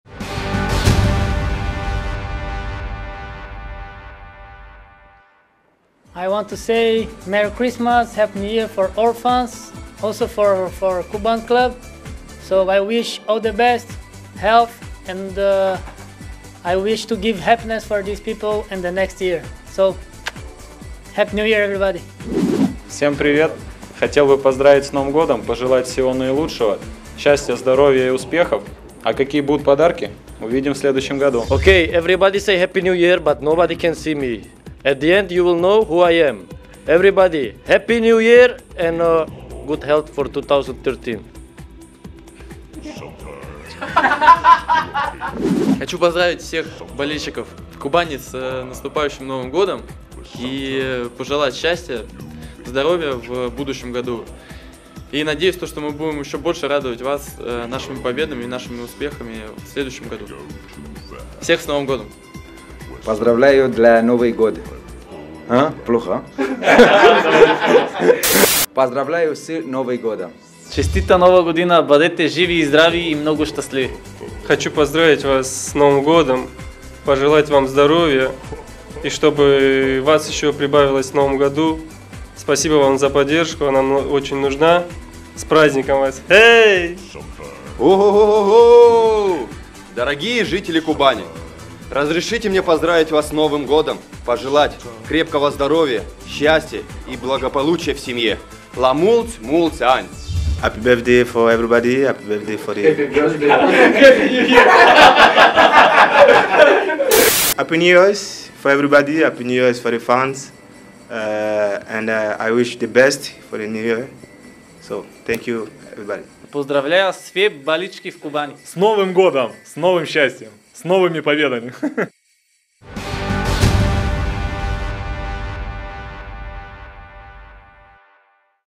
Новогоднее поздравление футболистов "Кубани"
Желто - зеленые деды Морозы поздравляют всех болельщиков "Кубани" с Новым годом!